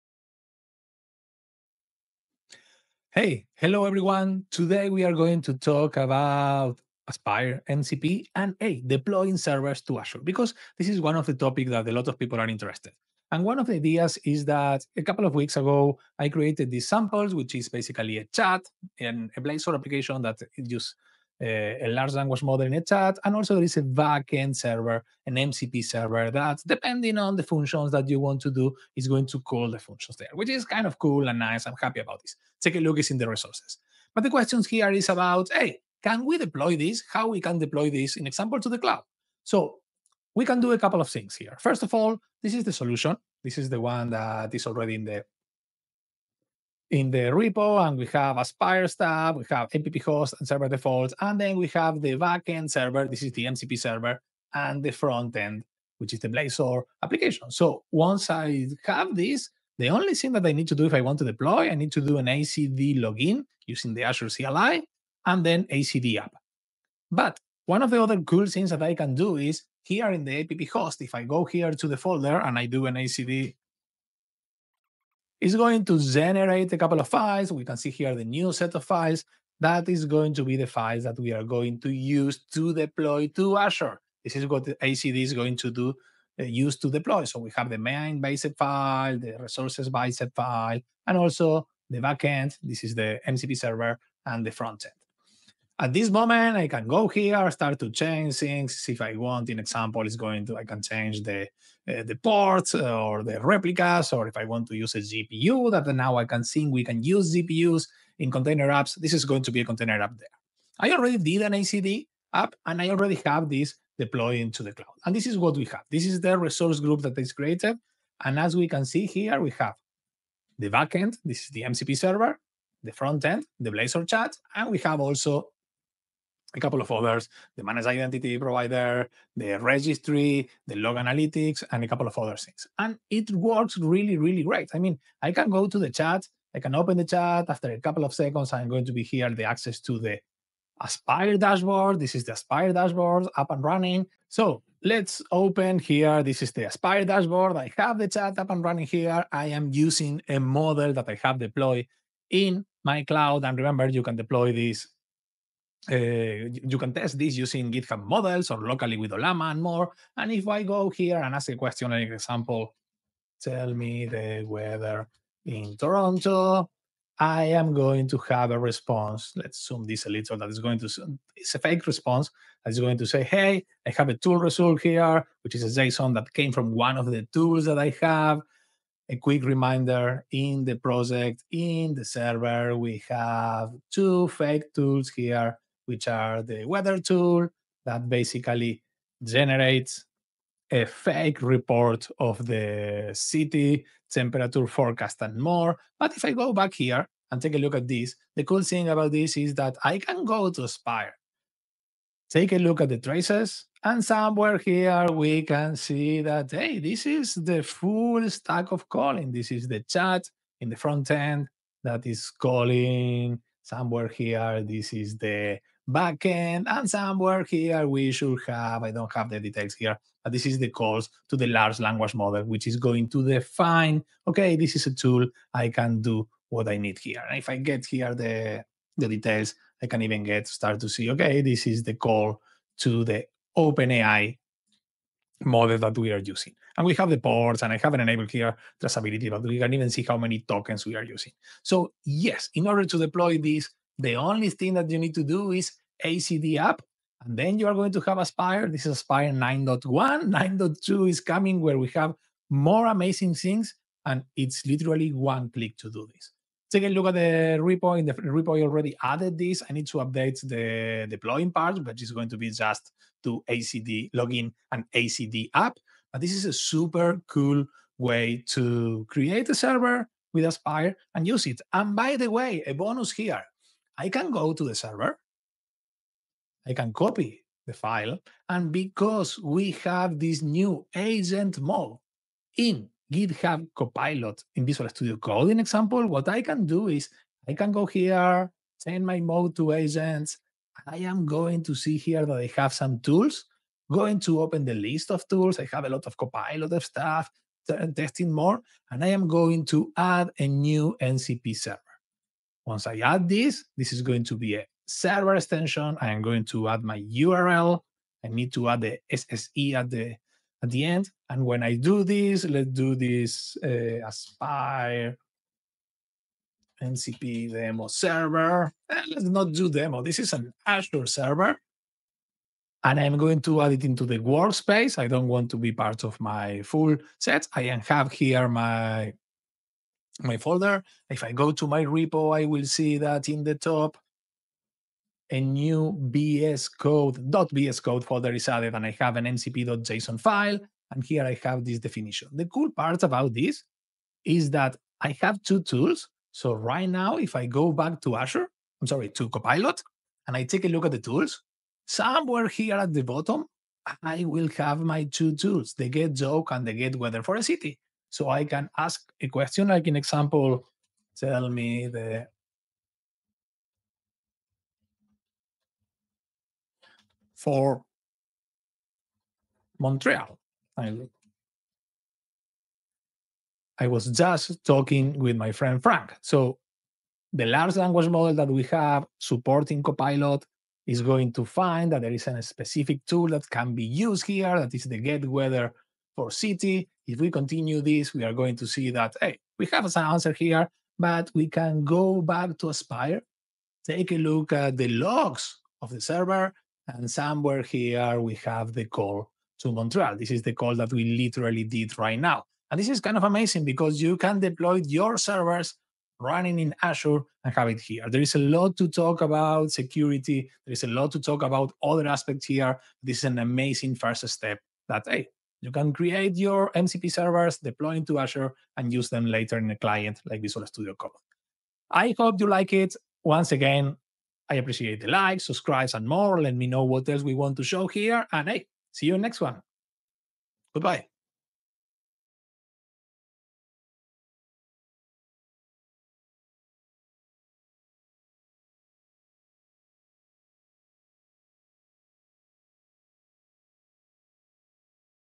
Deploy a .NET Aspire Chat App with MCP Tools to Azure
This is the second part of our .NET Aspire + MCP adventure! In this video, I’ll show you how to take things further by deploying a Blazor-based Chat UI and a MCP server with 2 custom tools used by the chat app.

📦 What’s inside:
-Setting up the Aspire project
-Wiring up the MCP Server
-Creating tools used by the Chat App
-Running the full solution with everything connected

🧠 Perfect if you're experimenting with:
-Local AI agents
-Chat UI on Blazor
-.NET Aspire & app orchestration
-Custom dev tools in MCP